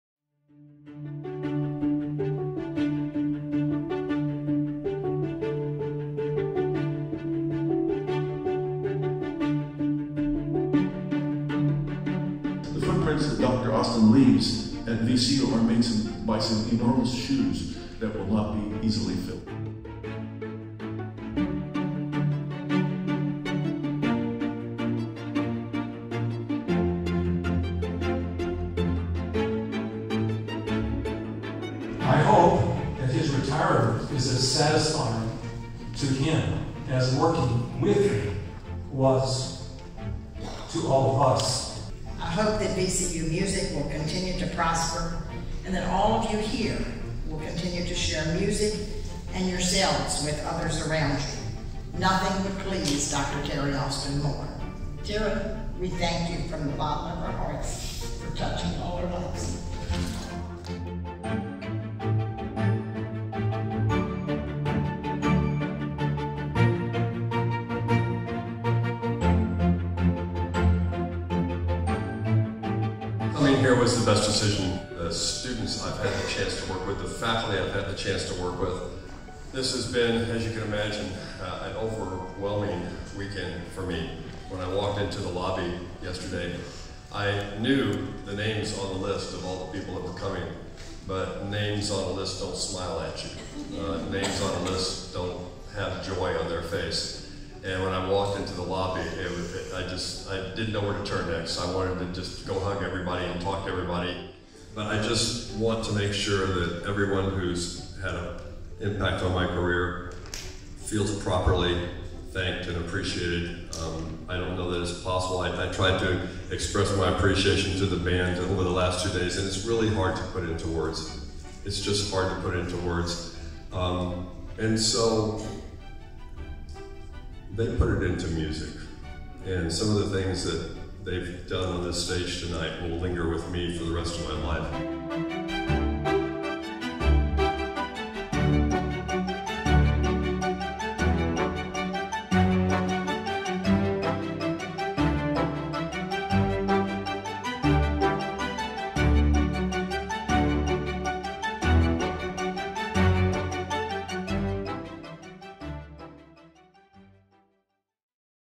Terry Austin, Ph.D. retirement concert
Terry Austin's friends, colleagues and former students celebrated his 36-year career at VCU with a concert.